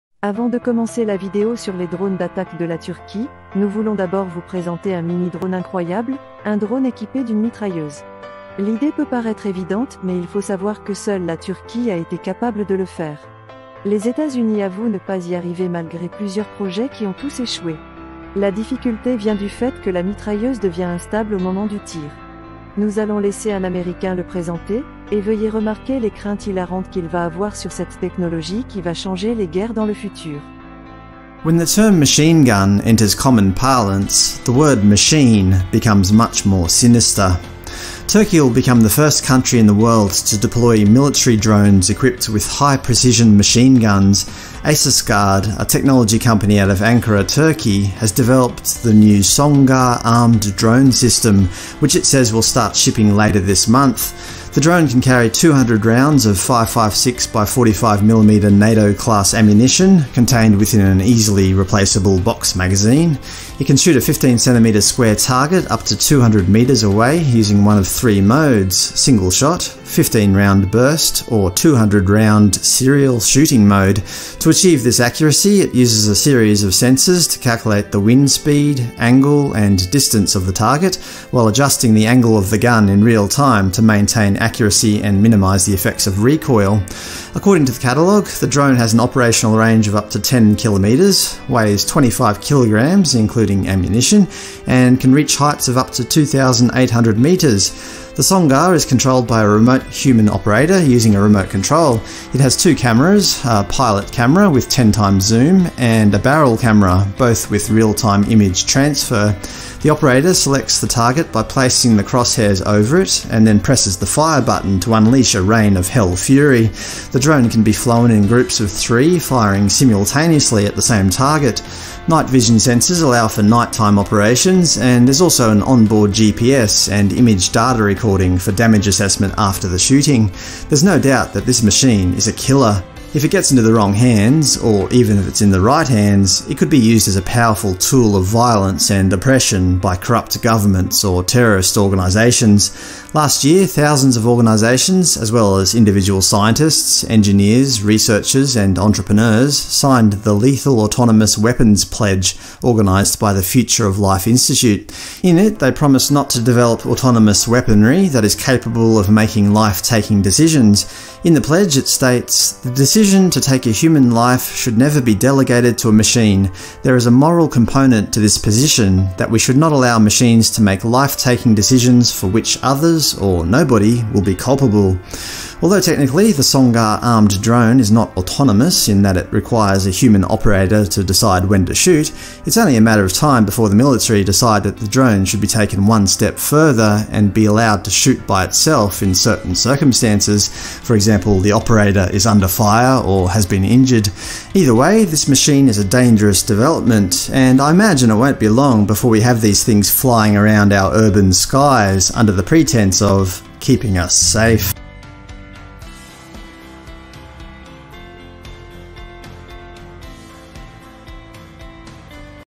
Drone Turc|Premier Drone Mitrailleuse Au Monde-Les Américains Avouent Ne Pas Y Arriver
Le premier drone muni d'une mitrailleuse dans le monde,le drone SONGAR conçue par la Turquie. Les États-Unis avouent ne pas y arriver . Une révolution dans la technologie militaire qui va changer à jamais les guerres du futur.